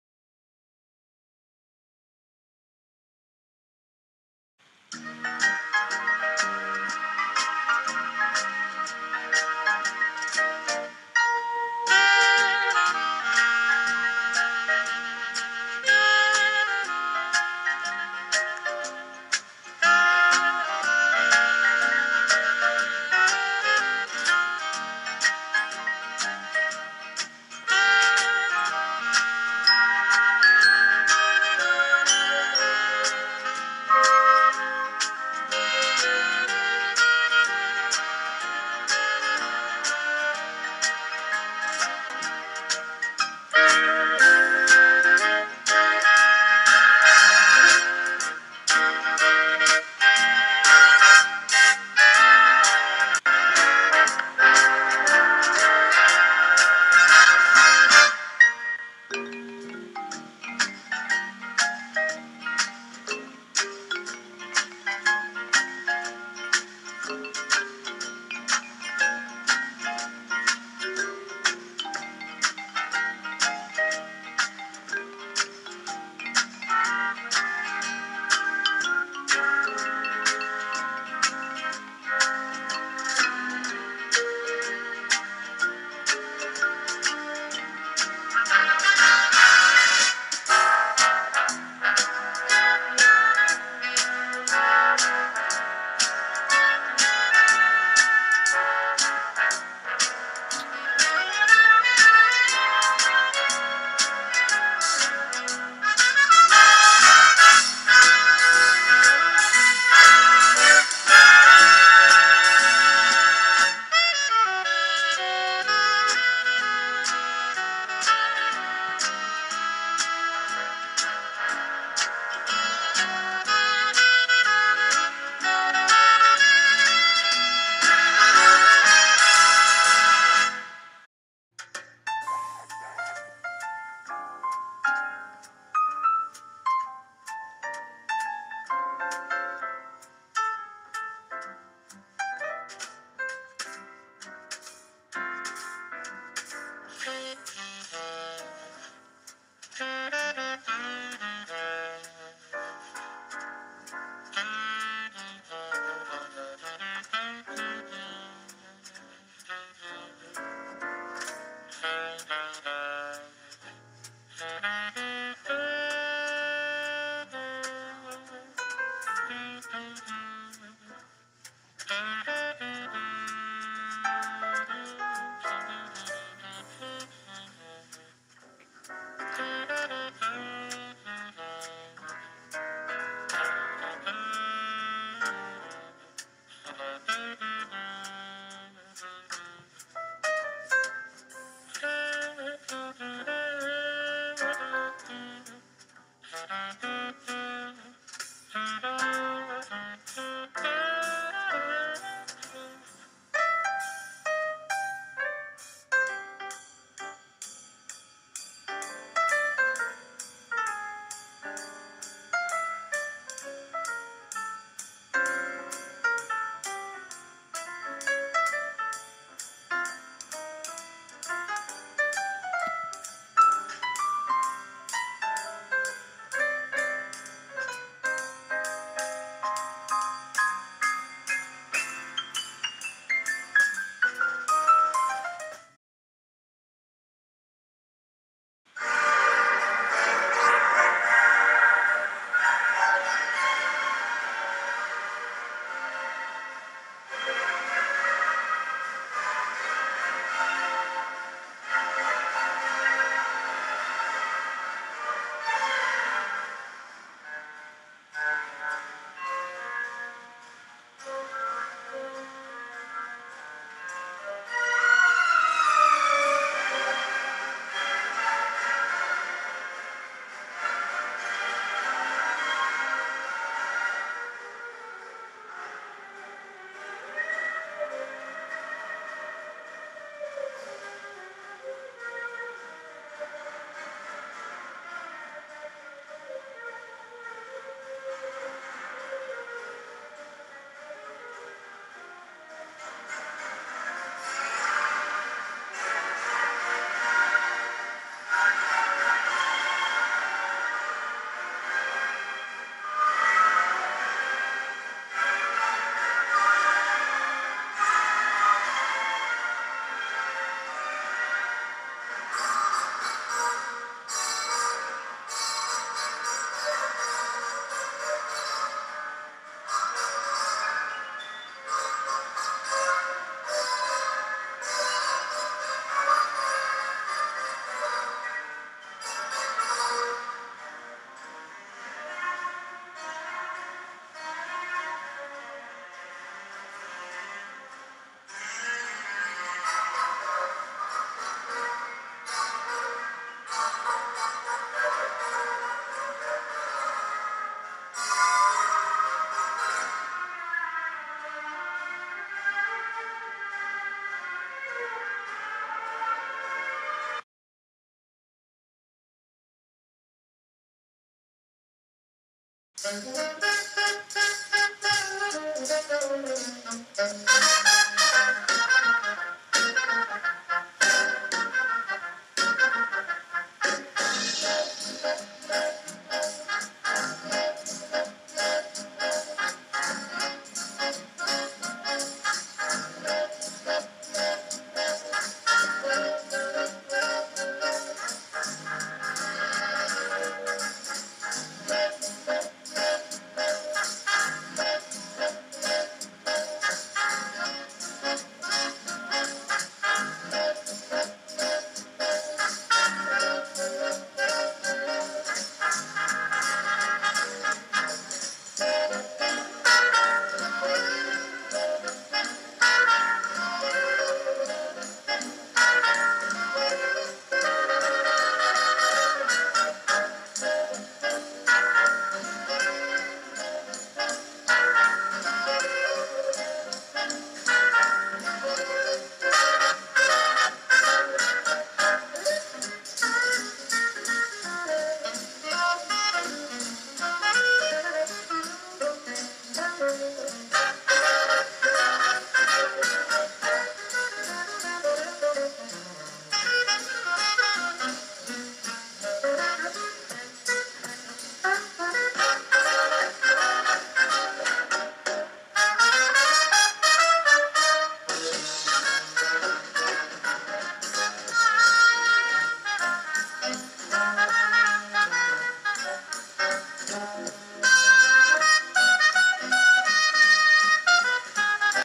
A voyage on Iconic
Recorded on MV Iconic's second New York-Southampton crossing on July 7-9 1936(September 2018)